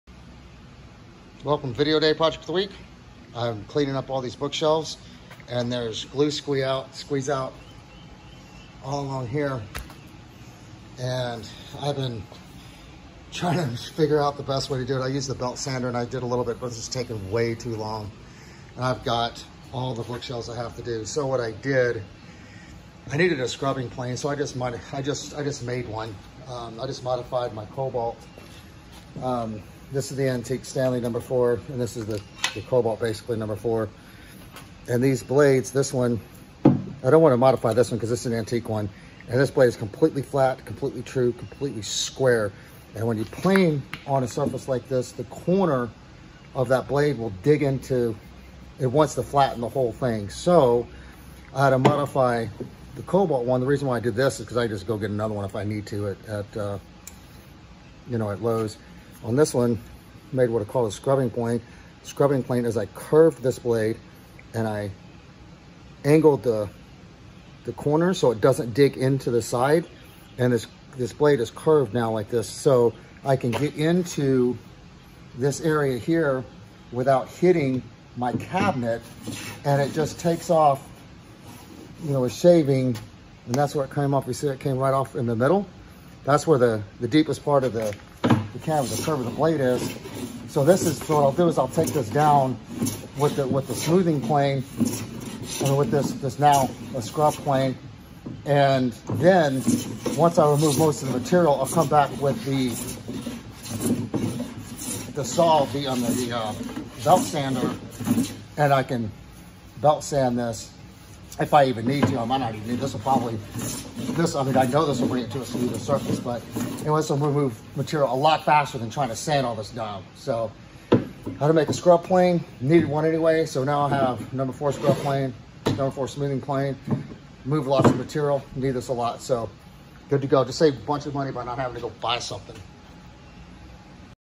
Made a Scrub Plan
Mortified a Lowe’s smoothing plane into a scrub plane. I’ve needed one for years and decided to make one. Couldn’t be more happy with its performance.